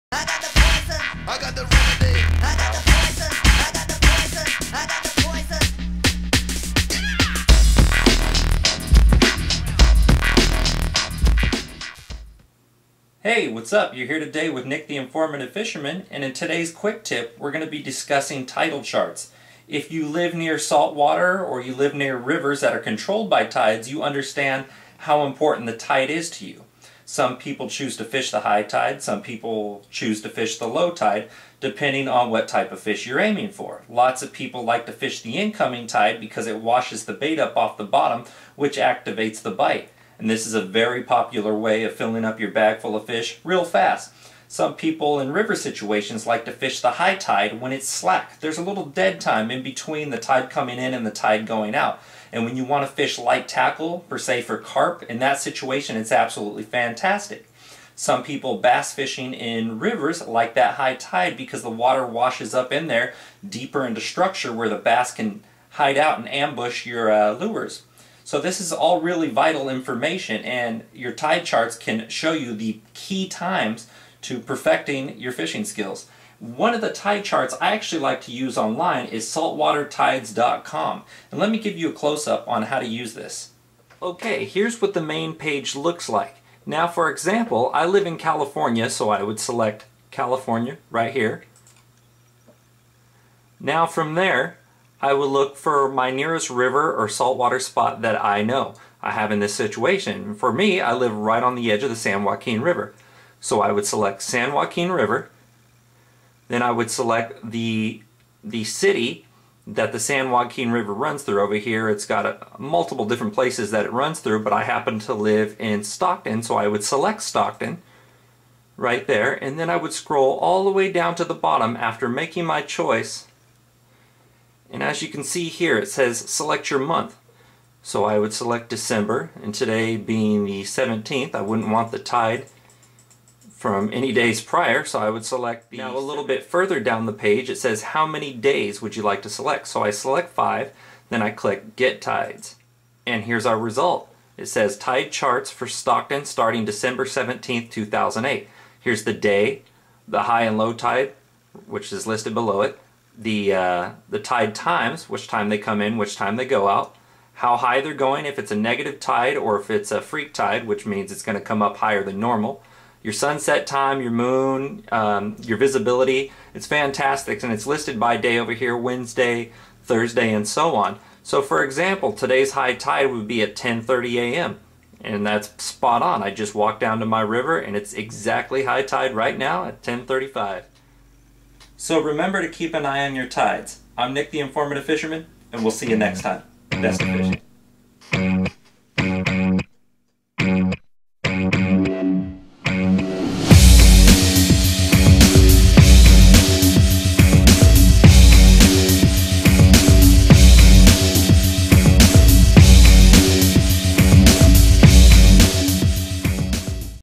Tide Charts
This video covers the understanding of tide charts & the benefits from using them.